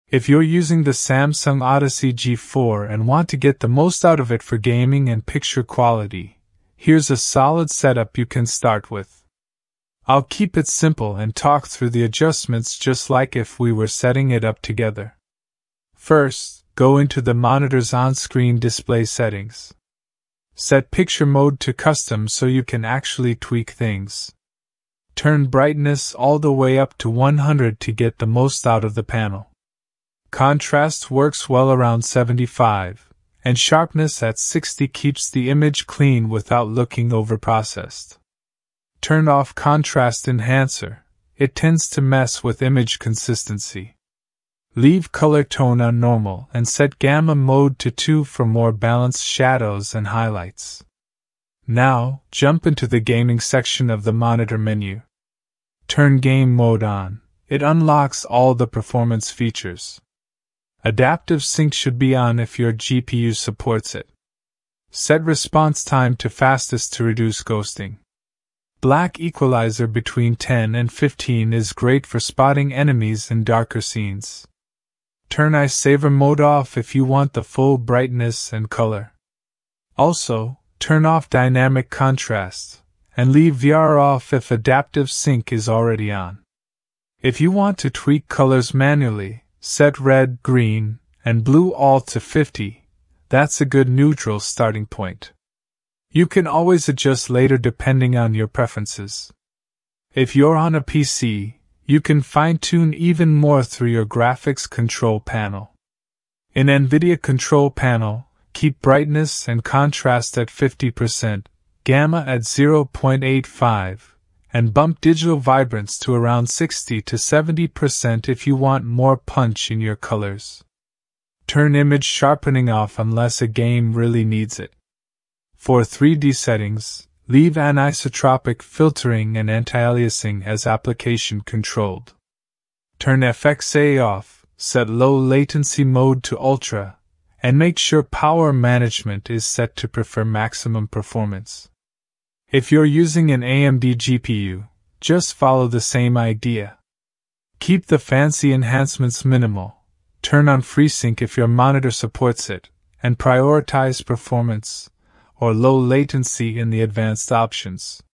Best Settings For Samsung Odyssey G4
Discover the ultimate settings for your Samsung Odyssey G4 monitor in this comprehensive guide. Whether you're a gamer seeking the best performance or a professional looking for accurate color representation, we cover everything you need to know. From adjusting brightness and contrast to optimizing refresh rates and response times, this video will help you unlock the full potential of your display. Join us as we walk you through each setting step-by-step, ensuring you achieve the best visual experience possible.

Thank you for your support!

 Explore the Best Deals
► If you are purchasing from Amazon, you can use this